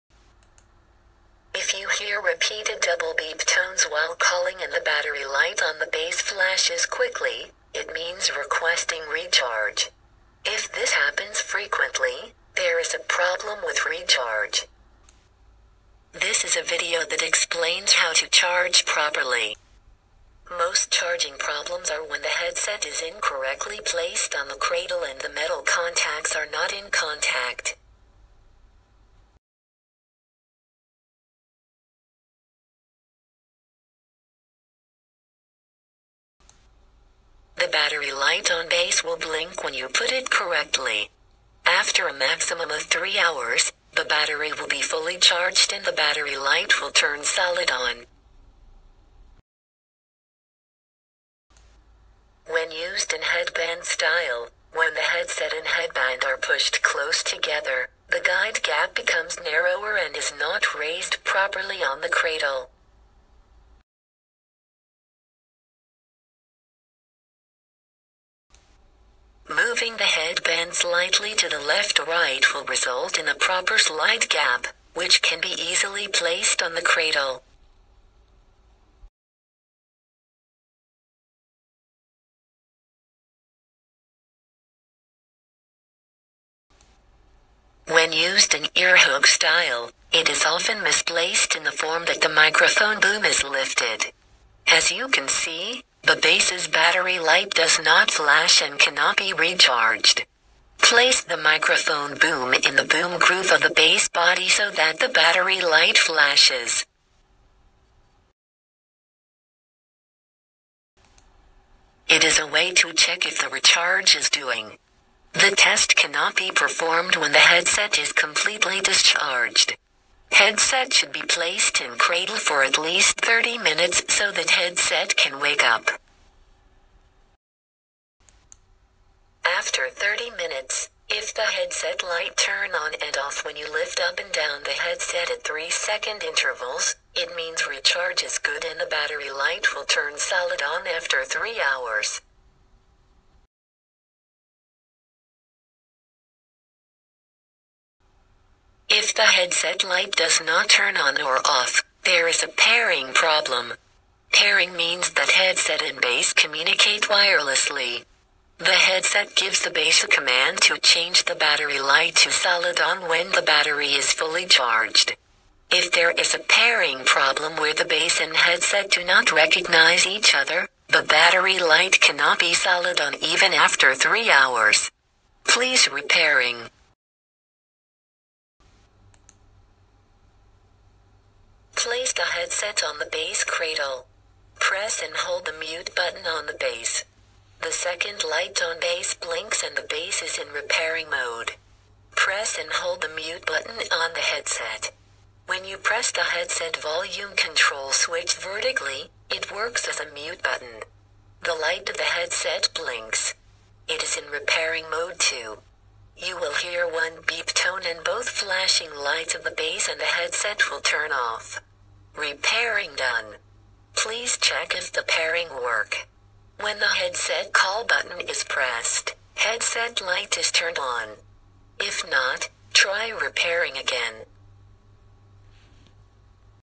INNOTALK Wireless Headset ReCharging
This is a video that you can refer to when the INNOTALK Wireless headset is not charging well or when there is a problem with less time to use.